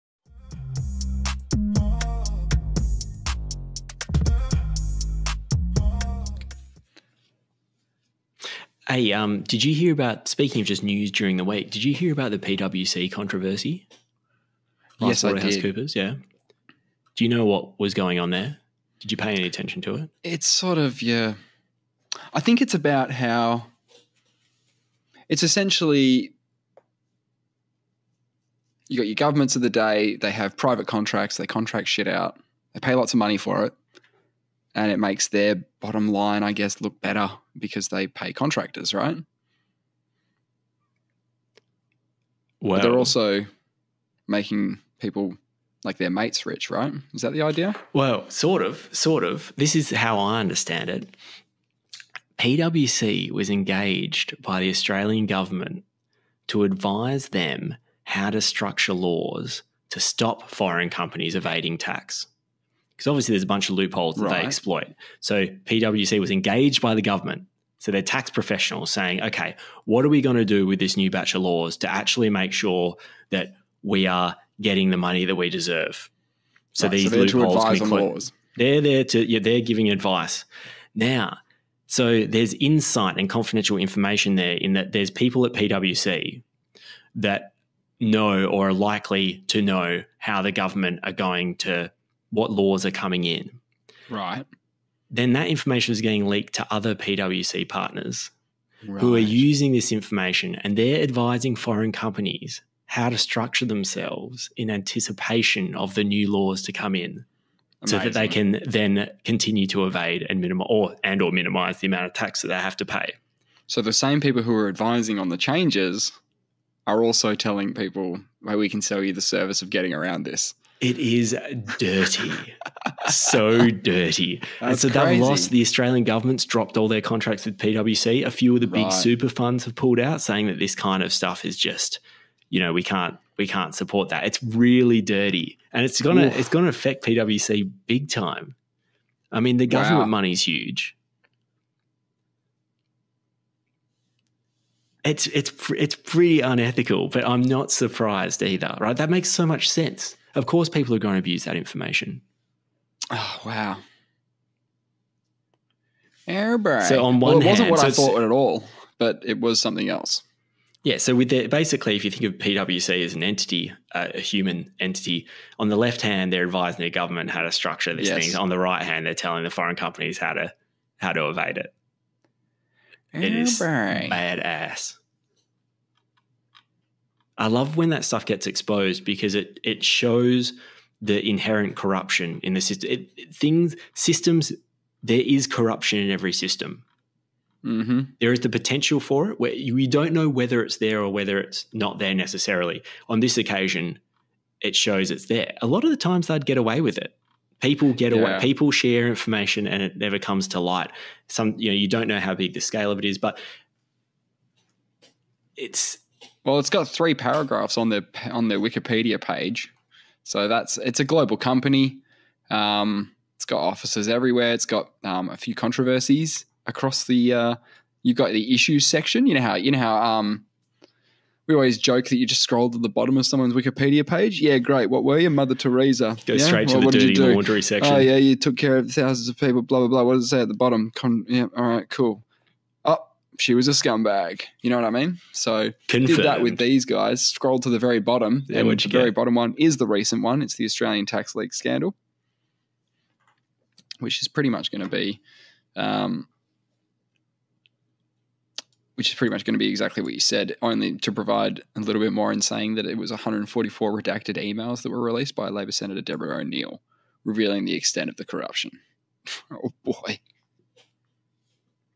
PWC controversy
Val and Nate discuss the PWC controversy in a vacuous manner.

Extract from Episode 53.